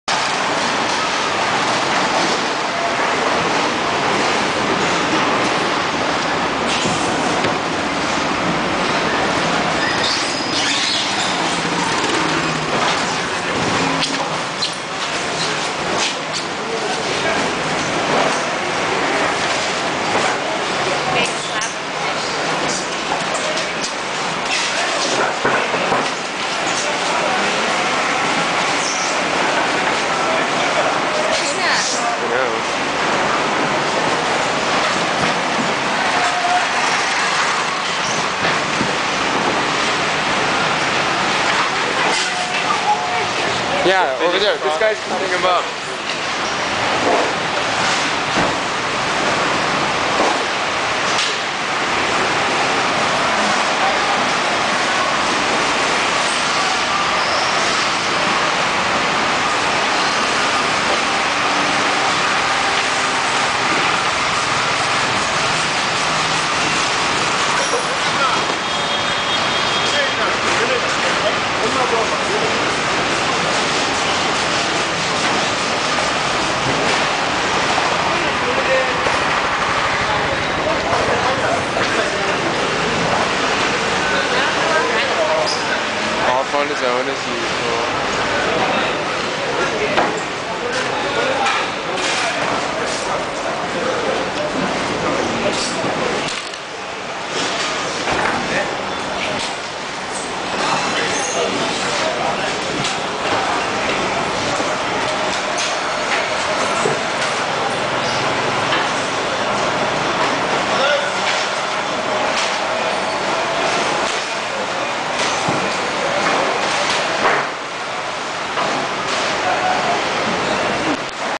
Tsukiji Fish Market
It's hard to describe the world's oldest fish market. "Organized chaos" probably best sums it up.